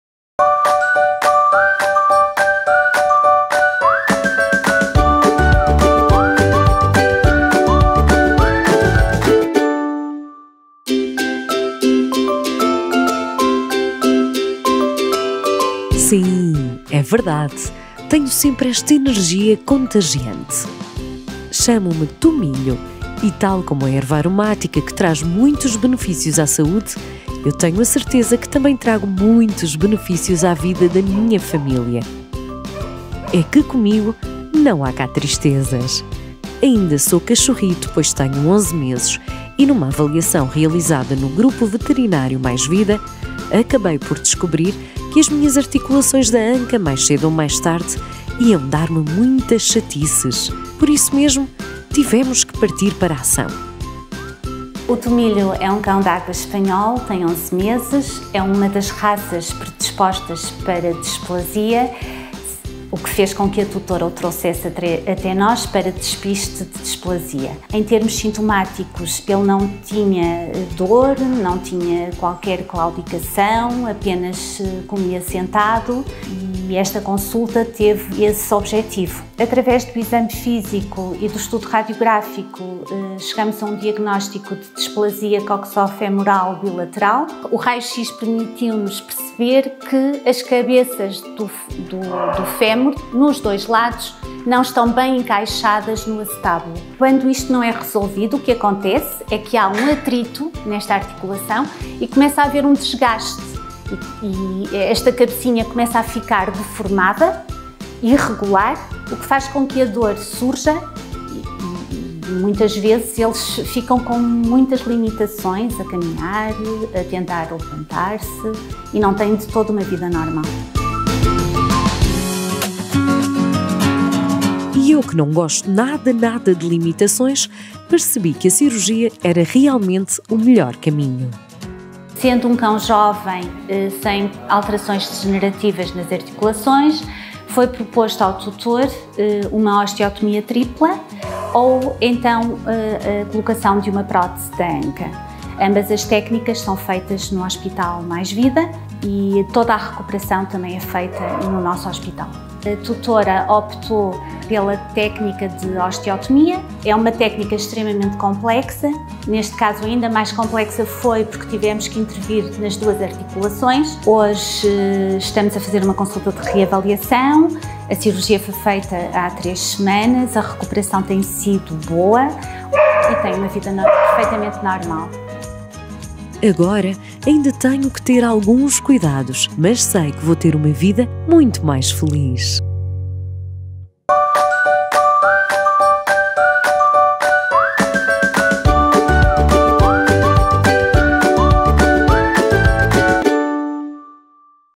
Tomilho, o cão de água com displasia da anca | O Meu Melhor Amigo
• É isso mesmo que leram! O amiguinho deste episódio chama-se Tomilho e é um cachorrinho com energia para dar e vender. 🐶✨
Aos 11 meses, numa avaliação realizada no Grupo “Mais Vida”, descobriram que as articulações da sua anca mais cedo ou mais tarde iriam limitá-lo em algumas brincadeiras, e por isso a melhor solução seria ser operado. 🩺👨🏻‍⚕

• Neste episódio de “O Meu Melhor Amigo” saiba como o Grupo “Mais Vida” possiblitou ao Tomilho um futuro sem limitações e uma vida muito mais feliz. 🥰

🧑🏻‍⚕️| Drª. Sandrina Pinto
🐶| Tomilho
🏥| Grupo Veterinário MaisVida

📹| Operador de Câmara:  João Gabriel
👨‍💻| Edição e Pós Produção: João Gabriel